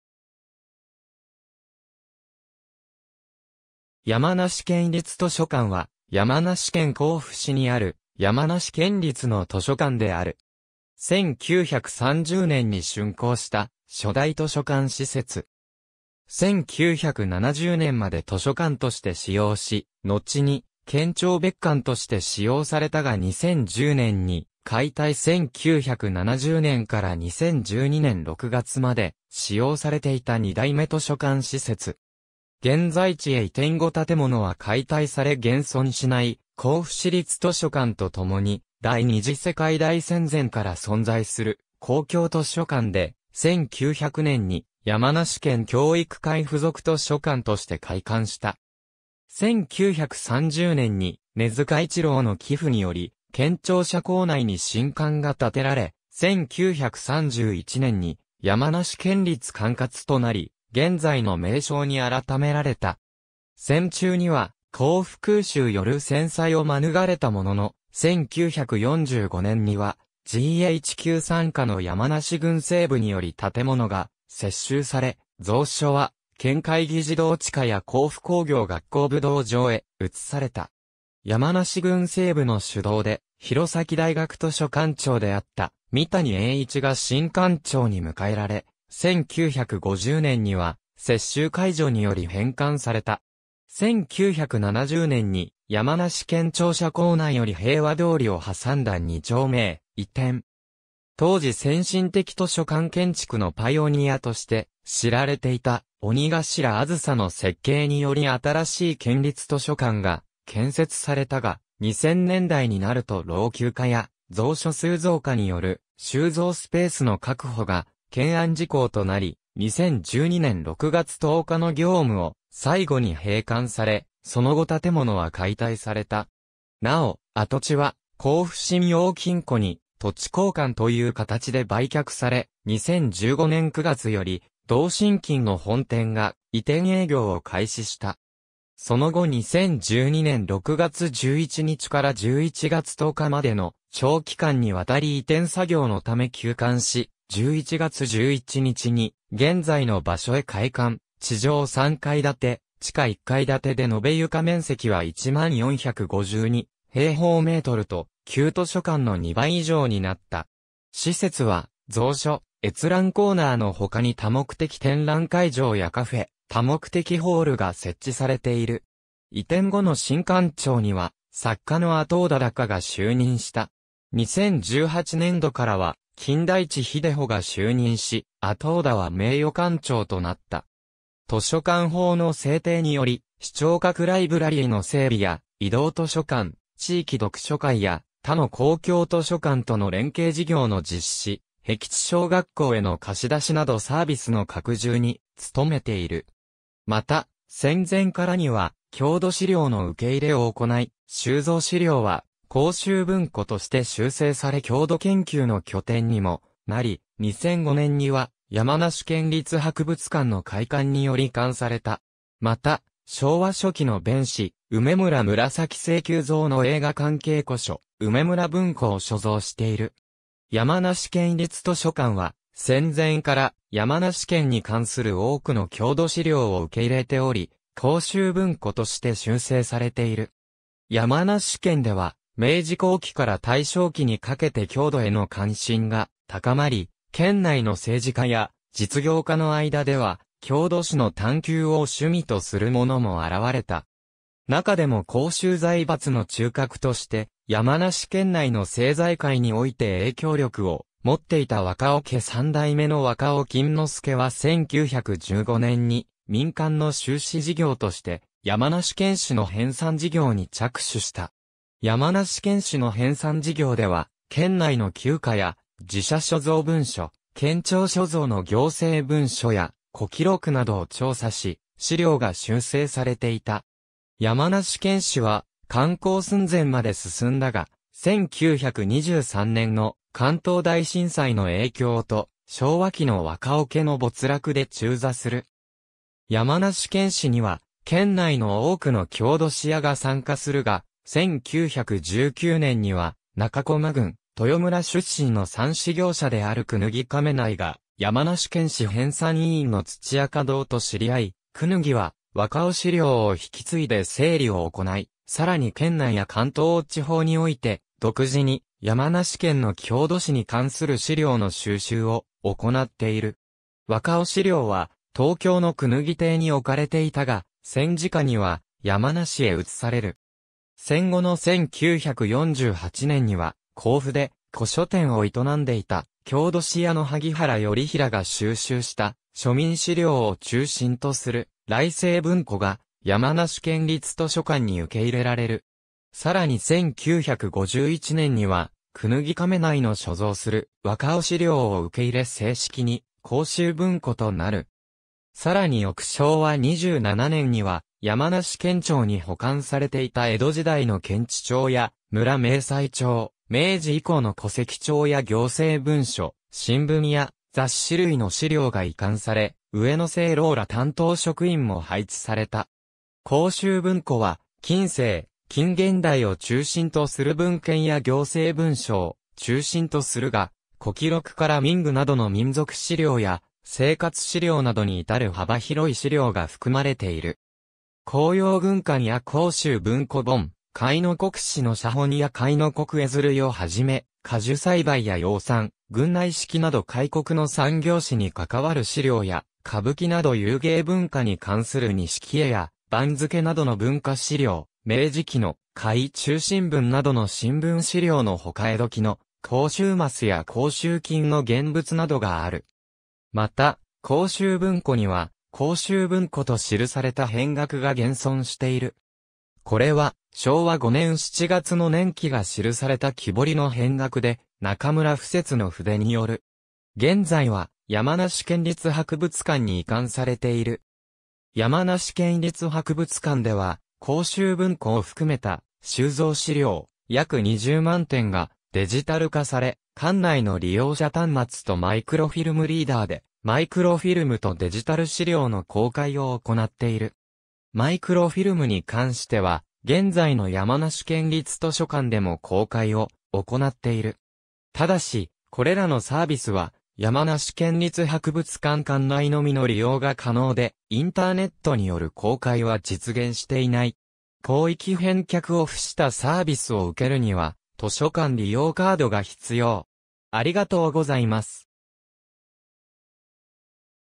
山梨県立図書館
山梨県立図書館（やまなしけんりつとしょかん）は、山梨県甲府市にある山梨県立の図書館である。
1930年（昭和5年）に竣工した初代図書館施設。
1970年まで図書館として使用し、のちに県庁別館として使用されたが2010年（平成22年）に解体 1970年から2012年6月まで使用されていた2代目図書館施設。
現在地へ移転後建物は解体され現存しない （2007年12月30日撮影） 甲府市立図書館とともに第二次世界大戦前から存在する公共図書館で、1900年（明治33年）に山梨県教育会付属図書館として開館した。
1930年（昭和5年）に根津嘉一郎の寄付により県庁舎構内に新館が建てられ、1931年（昭和6年）に山梨県立管轄となり、現在の名称に改められた。
戦中には甲府空襲よる戦災を免れたものの、1945年（昭和20年）にはGHQ傘下の山梨軍政部により建物が接収され、蔵書は県会議事堂地下や甲府工業学校武道場へ移された。
山梨軍政部の主導で弘前大学図書館長であった三谷栄一が新館長に迎えられ、1950年（昭和25年）には接収解除により返還された。
1970年（昭和45年）に山梨県庁舎構内より平和通りを挟んだ二丁目へ移転（それまでの建物は山梨県庁舎第一南別館として2010年まで使用され、現在は改築され防災新館となっている）。
当時先進的図書館建築のパイオニアとして知られていた鬼頭梓の設計により新しい県立図書館が建設されたが、2000年代になると老朽化や蔵書数増加による収蔵スペースの確保が懸案事項となり、2012年6月10日の業務を最後に閉館され、その後建物は解体された。
なお、跡地は甲府信用金庫に土地交換という形で売却され、2015年9月より同信金の本店が移転営業を開始した。
その後2012年6月11日から11月10日までの長期間にわたり移転作業のため休館し、11月11日に現在の場所へ開館、地上3階建（一部4階建）、地下1階建で延床面積は10,452m2と、旧図書館（4,249m2）の2倍以上になった。
施設は蔵書・閲覧コーナーのほかに多目的展覧会場やカフェ、多目的ホールが設置されている。
移転後の新館長には作家の阿刀田高が就任した。
2018年度からは、金田一秀穂が就任し、阿刀田は名誉館長となった。
図書館法の制定により視聴覚ライブラリーの整備や移動図書館、地域読書会や他の公共図書館との連携事業の実施、僻地小学校への貸し出しなどサービスの拡充に努めている。
また、戦前からには郷土資料の受け入れを行い、収蔵資料は「甲州文庫」として集成され郷土研究の拠点にもなり、2005年には山梨県立博物館の開館により移管された。
また、昭和初期の弁士梅村紫声旧蔵の映画関係古書「梅村文庫」を所蔵している。
山梨県立図書館は戦前から山梨県に関する多くの郷土資料を受け入れており、「甲州文庫」として集成されている。
山梨県では明治後期から大正期にかけて郷土への関心が高まり、県内の政治家や実業家の間では郷土史の探求を趣味とするものも現れた。
中でも甲州財閥の中核として山梨県内の政財界において影響力を持っていた若尾家三代目の若尾謹之助は1915年（大正4年）に民間の修史事業として『山梨県志』の編纂事業に着手した。
「山梨県志」の編纂事業では県内の旧家や寺社所蔵文書、県庁所蔵の行政文書や古記録などを調査し、史料が集成されていた（後の「若尾資料」）。
「山梨県志」は刊行寸前まで進んだが、1923年（大正12年）の関東大震災の影響と、昭和期の若尾家の没落で中座する。
「山梨県志」には県内の多くの郷土史家が参加するが、1919年（大正8年）には中巨摩郡豊村（南アルプス市上今井、旧同郡櫛形町）出身の蚕糸業者である功刀亀内が「山梨県志」編纂委員の土屋夏堂と知り合い、功刀は「若尾資料」を引き継いで整理を行い、さらに県内や関東地方において独自に山梨県の郷土史に関する資料の収集を行っている。
若尾資料は東京の功刀邸に置かれていたが、戦時下には山梨へ移される。
戦後の1948年（昭和23年）には、甲府で古書店を営んでいた郷土史家の萩原頼平が収集した庶民資料を中心とする「頼生文庫」が山梨県立図書館に受け入れられる。
さらに1951年（昭和26年）には功刀亀内の所蔵する若尾資料を受け入れ正式に「甲州文庫」となる。
さらに翌昭和27年には山梨県庁に保管されていた江戸時代の検地帳や村明細帳、明治以降の戸籍帳や行政文書、新聞や雑誌類の資料が移管さ...